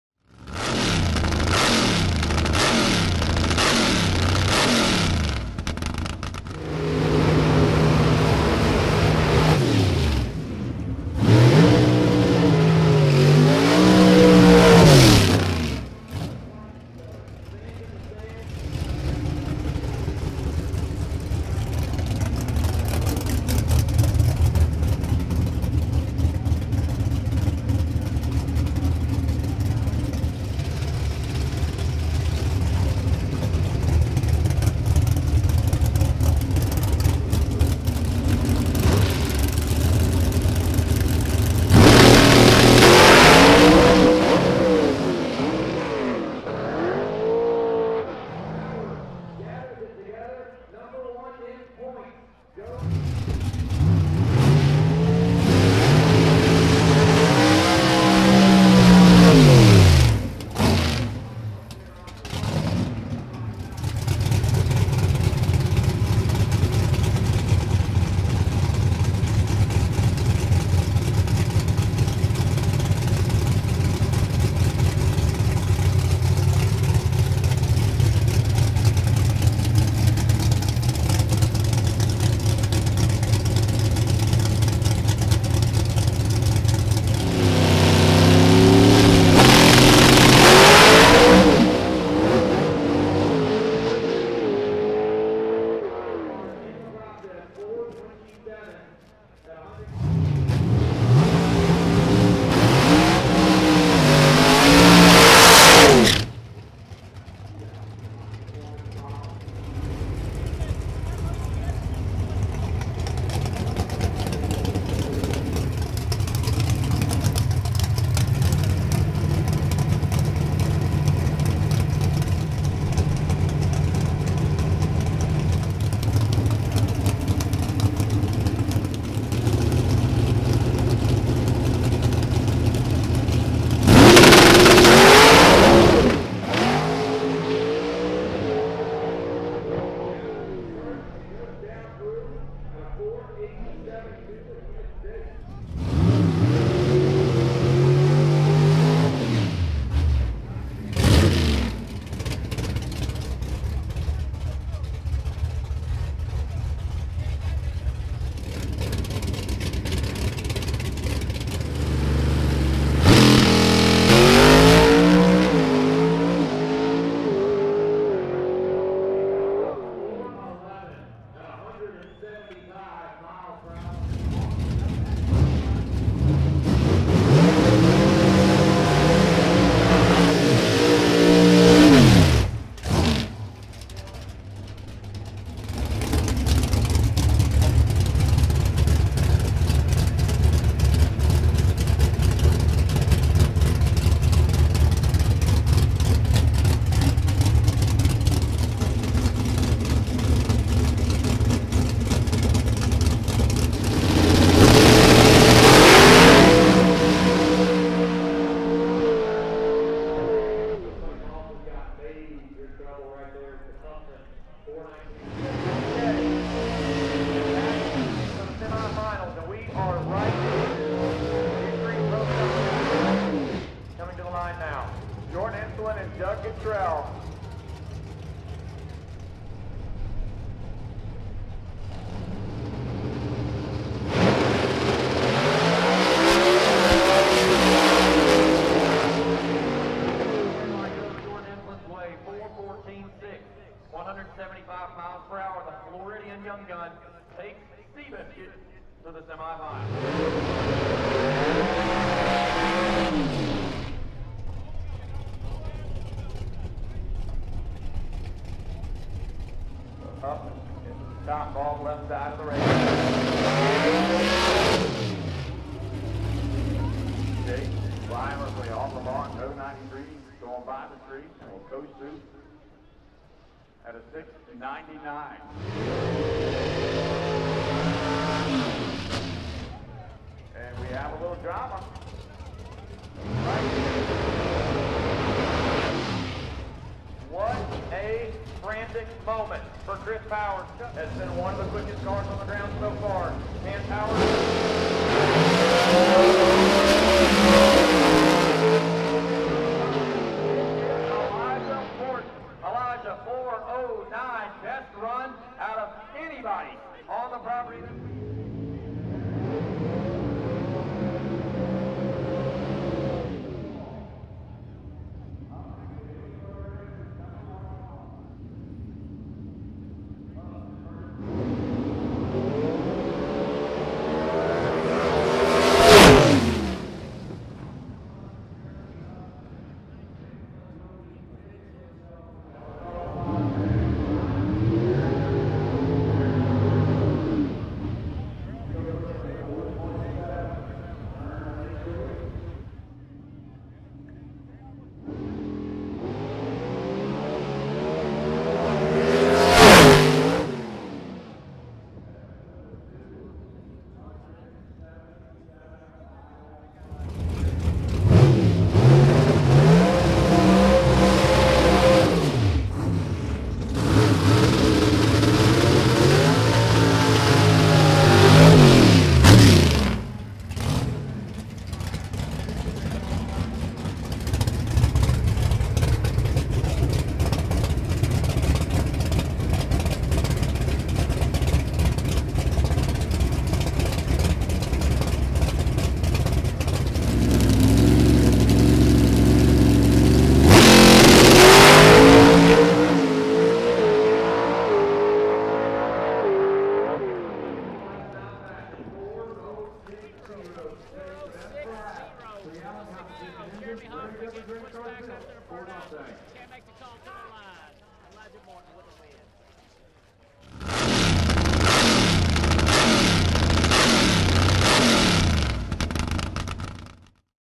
PDRA Extreme Pro Stock Eliminations - North vs South Shootout!
Elimination Coverage of Extreme Pro Stock From this past weekends PDRA North vs South Shootout at Maryland International Raceway!